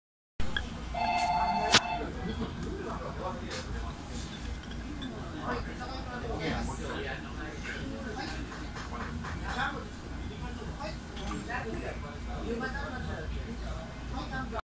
A rare fish （mudskipper's fish）
A rare fish. The name is a mudspringer. Sashimi